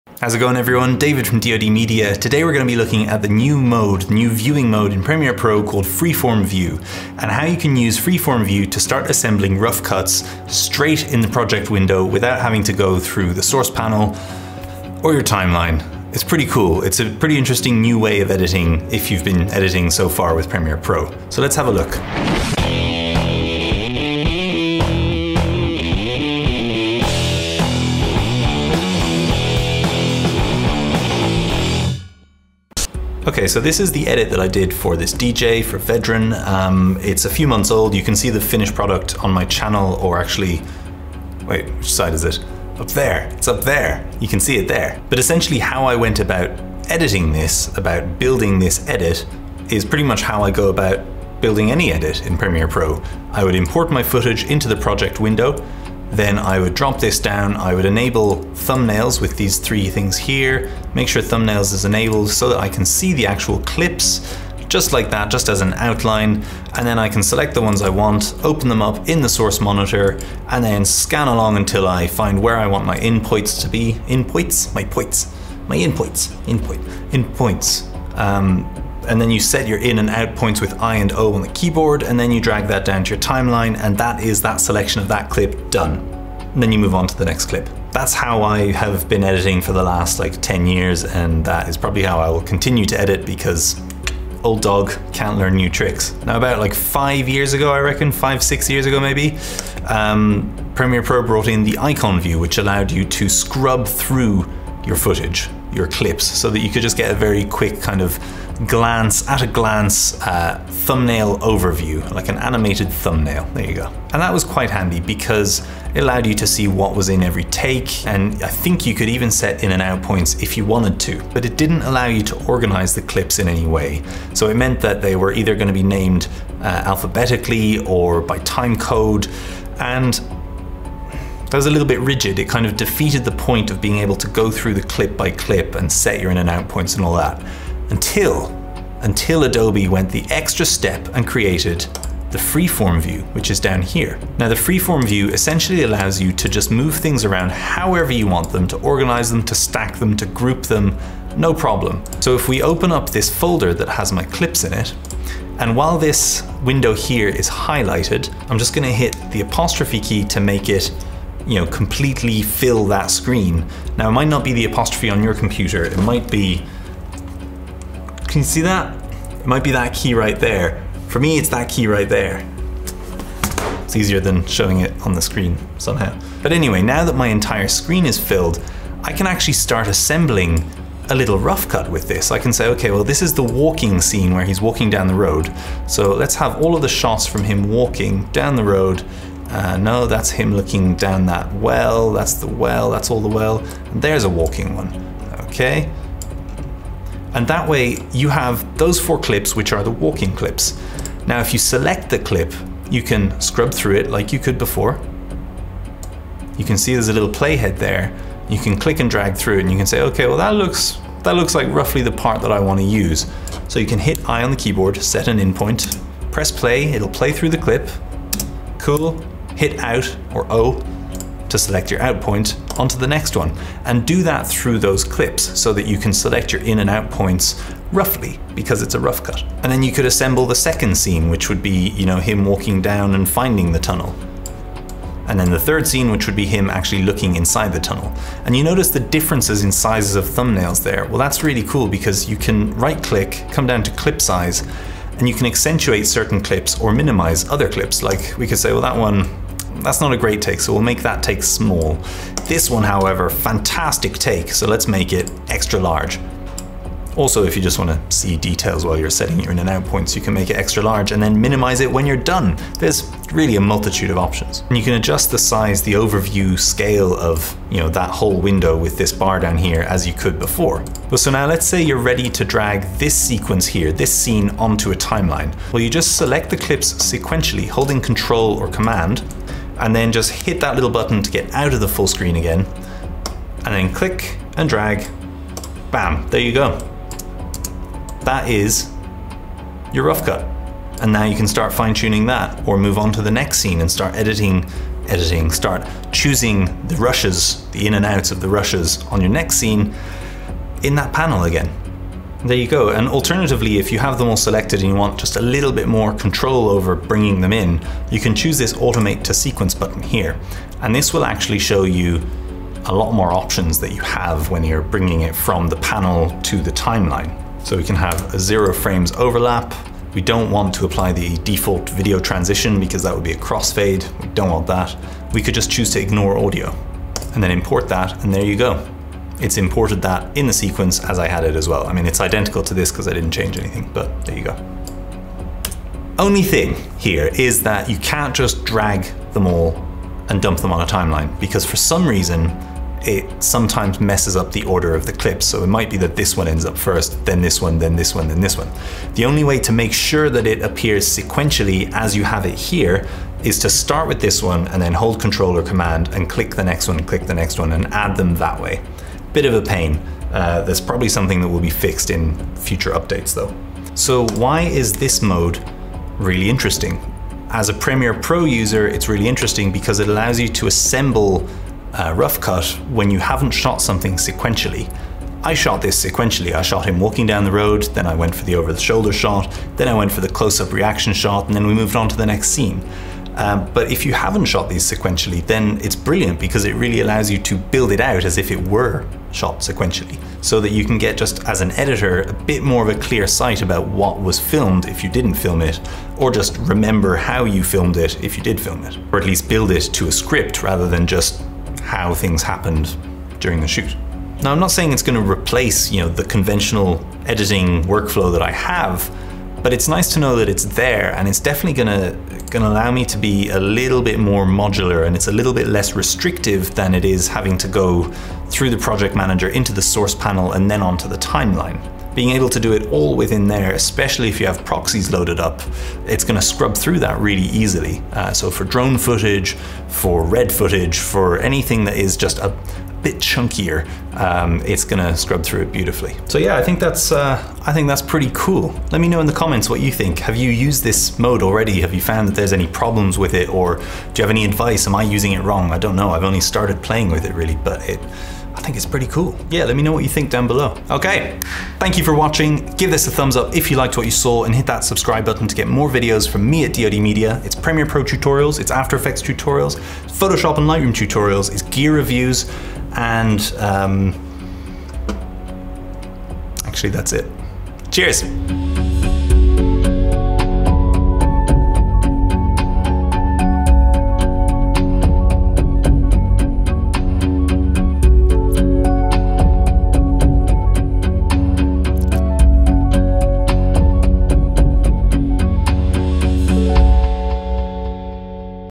Freeform View - New in Premiere Pro CC 2019
Checking out the Freeform View in Premiere Pro CC 2019

FILMMAKING GEAR
PHOTOGRAPHY GEAR

For product reviews, collaborations or partnerships, or to send me something nice, get in touch